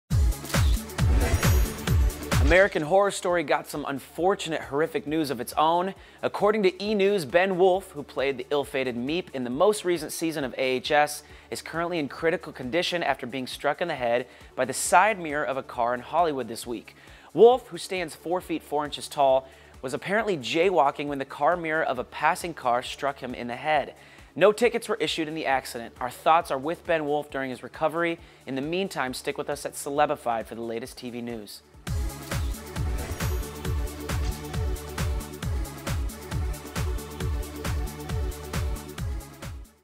American Horror Story Actor in Critical Condition! Ben Woolf aka "Meep" Struck by Car in Hollywood
American Horror Story Actor Ben Woolf in Critical Condition After Being Struck By Car Mirror in Hollywood

Ben Woolf, fondly known as "Meep" from American Horror Story: Freak Show, is currently recovering from a horrific experience of his own! The actor is in critical condition after jaywalking in Hollywood.

American Horror Story got some unfortunate horrific news of its own. According to E! News, Ben Woolf, who played the ill-fated Meep in the most recent season of AHS, is currently in critical condition after being struck in the head by the side mirror of a car in Hollywood this week. Woolf, who stands 4 feet 4 inches tall, was jaywalking when the car mirror of a passing car apparently struck him. No tickets were issued in the accident. Our thoughts are with Ben Woolf during his recovery. In the meantime, stick with us at Celebified for the latest TV news.